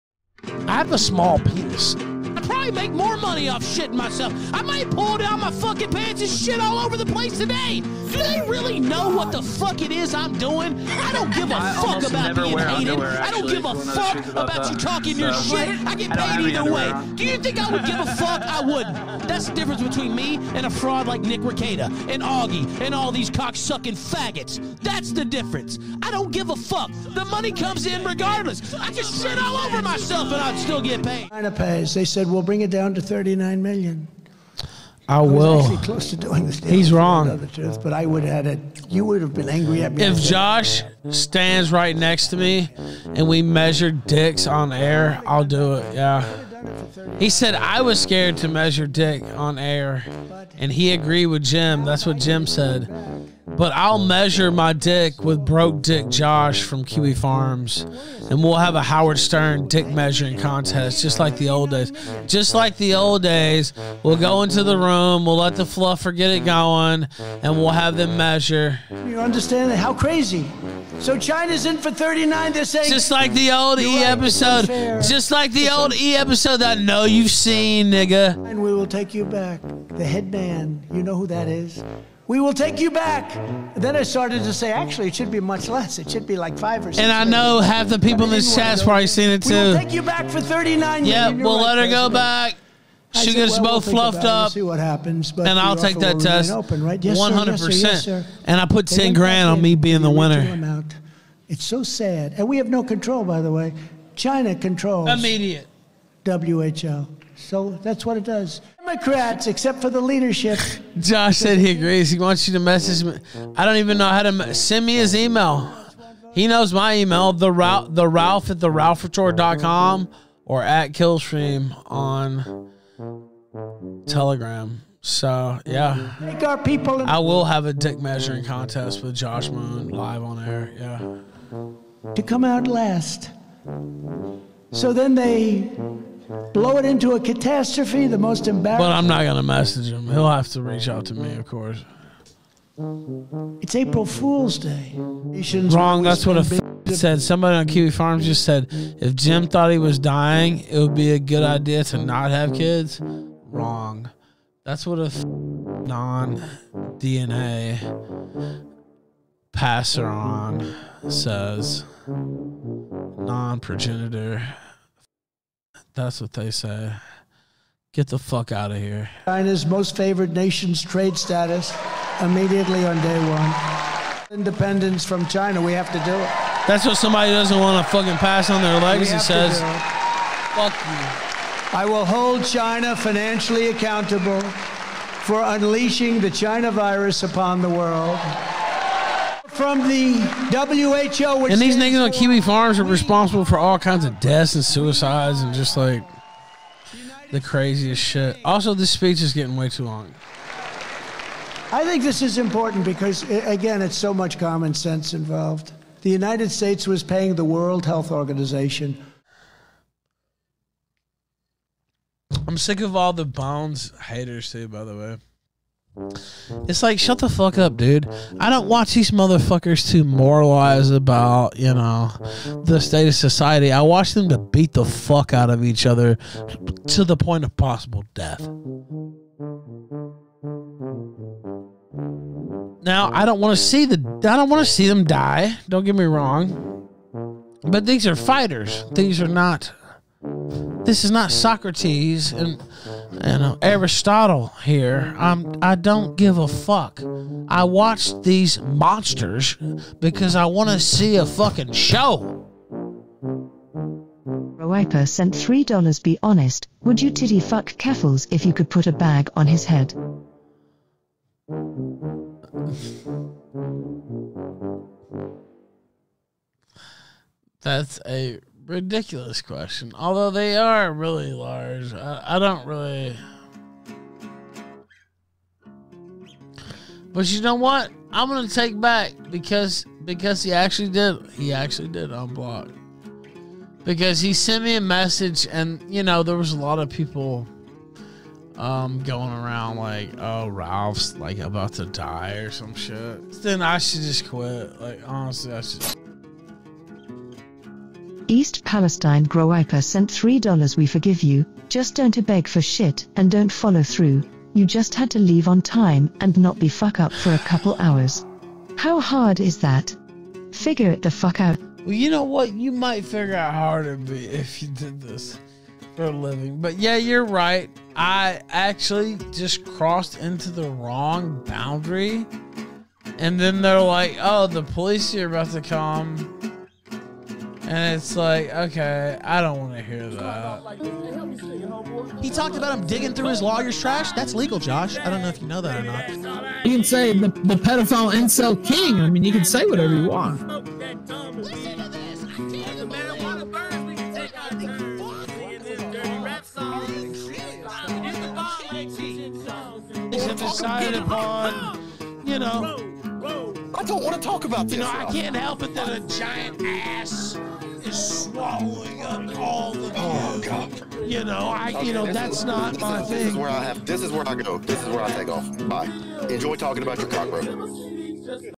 Ralph Challenges Josh Moon to a Literal Dick Measuring Contest
Ethan Ralph challenges Josh Moon, owner of Kiwifarms, to measure one another's penis. As long as Josh is standing right next to him, which is a strange stipulation to make.

Timecodes:
0:00 - Intro
0:30 - Ralph Challenges Josh Moon's Penis
2:25 - Ralph says you should have kids if you find out you're dying
4:14 - Groyper berates Ralph for missing flight to DC for CPAC
5:30 - Outro